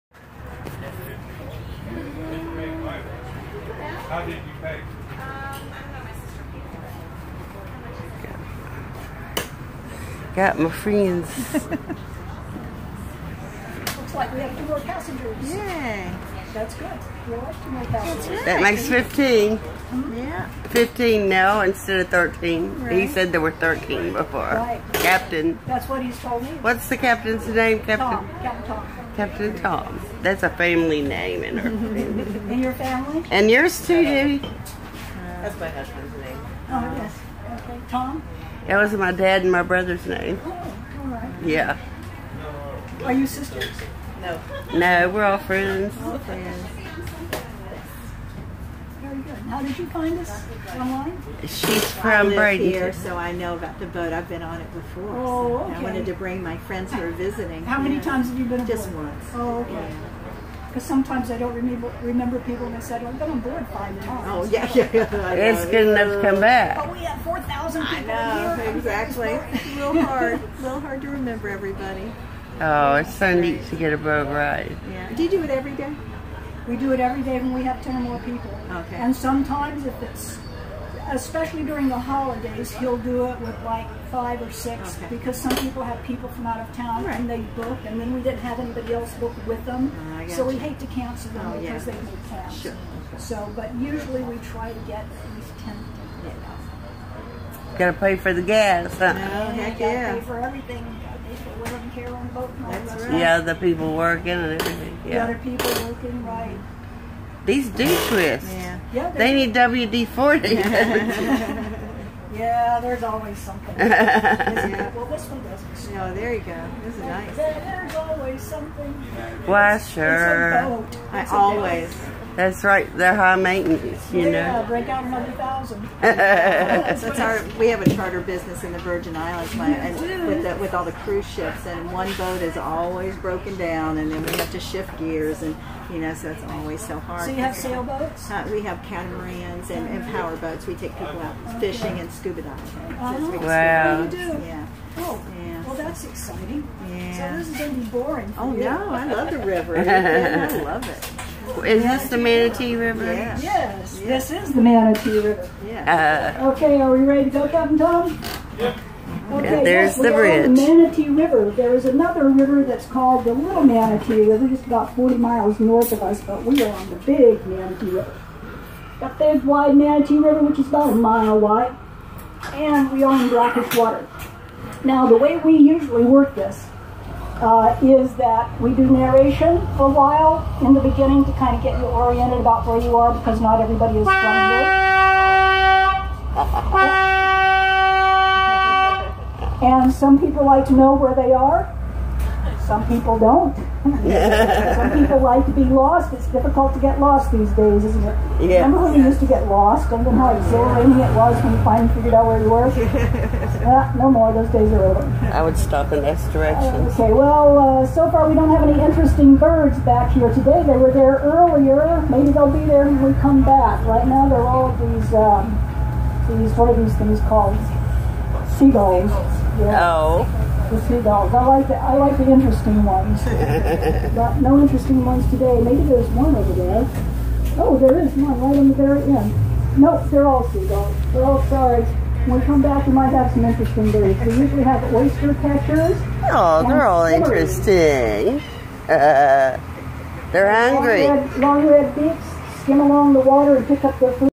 Adrain Smarr asked me to pet sit her sweet babies 12/28/19
Debbie's b'day Adraine